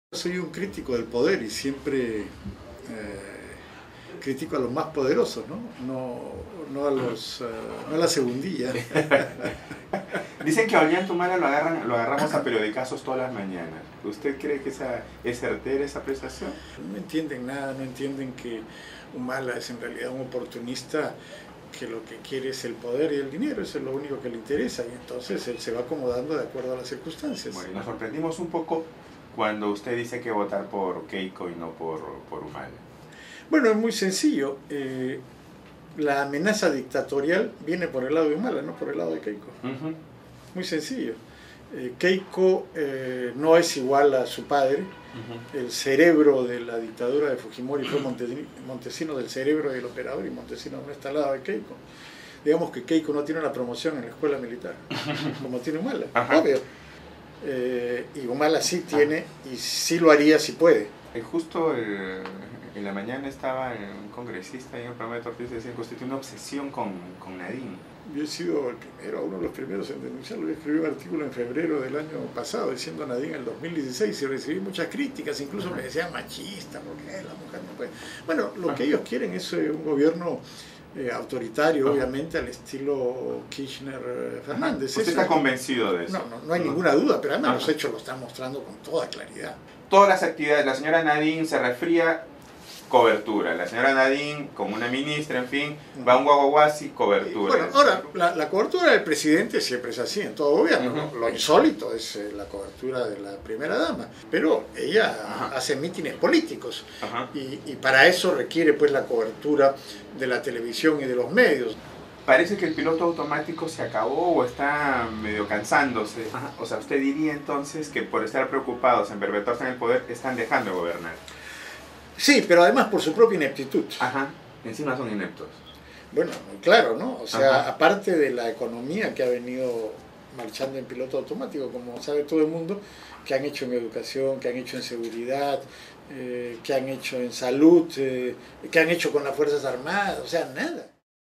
Fernando Rospigliosi: "Ollanta Humala, si pudiera, crearía una dictadura"
El analista político dice que la mayor parte de la izquierda no es democrática. Además, cree que Alejandro Toledo no fue un presidente corrupto y Alan García, sí.